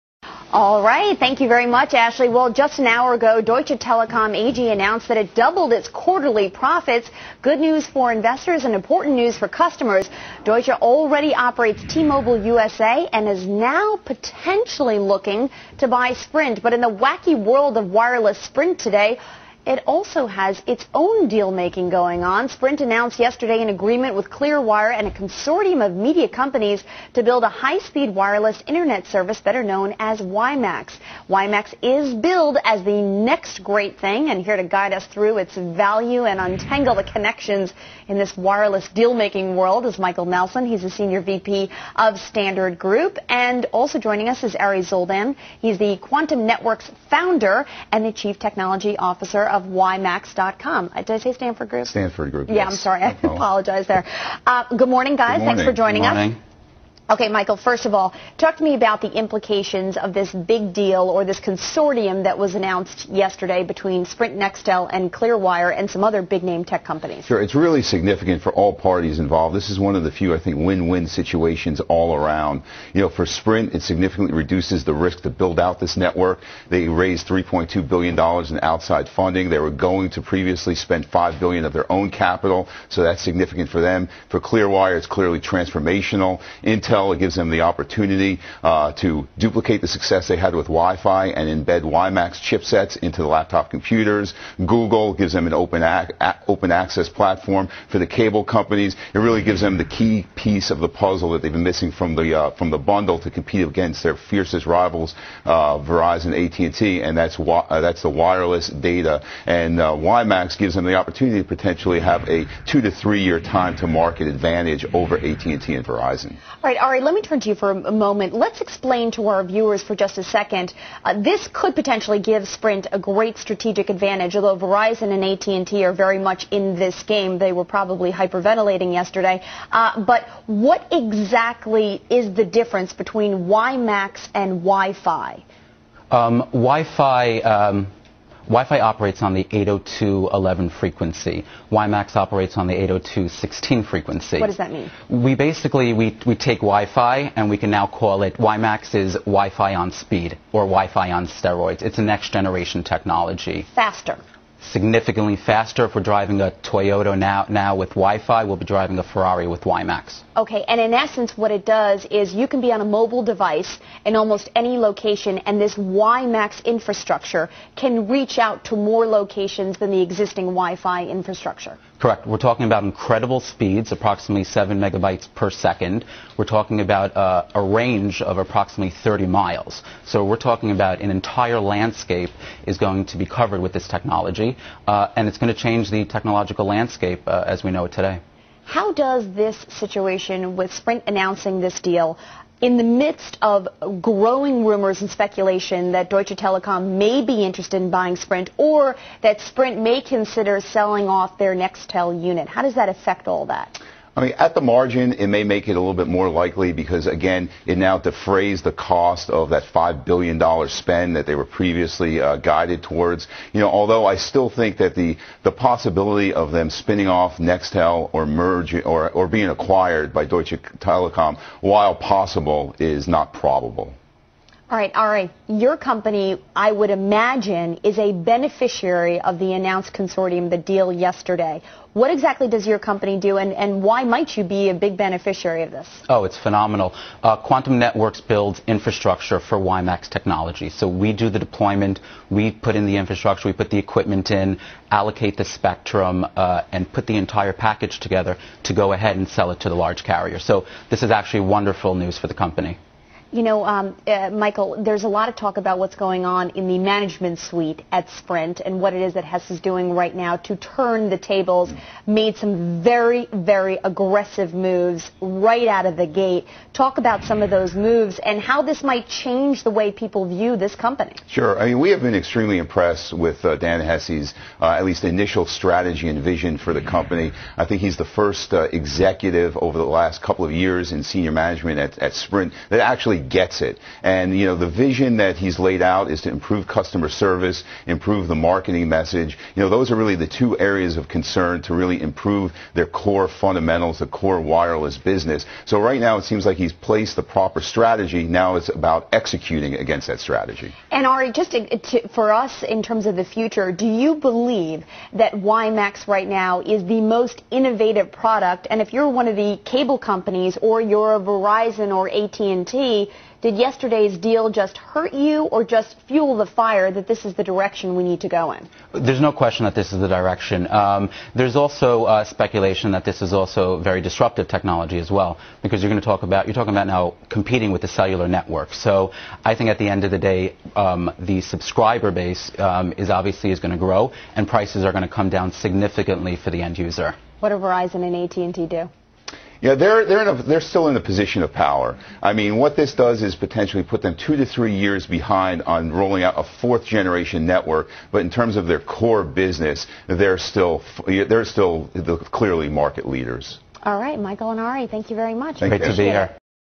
Ari Zoldan interviewed on Fox News about Wimax Technology
Interview of Ari Zoldan, the Wimax Expert and CEO of Quantum Networks, on Fox News. He is deeply involved into the Telecom and Emerging Technology Wimax and regularly interviewed about it on National TV.

If you want to learn more about the Wimax Technology, you can follow Ari Zoldan on his blog and check Wireless devices :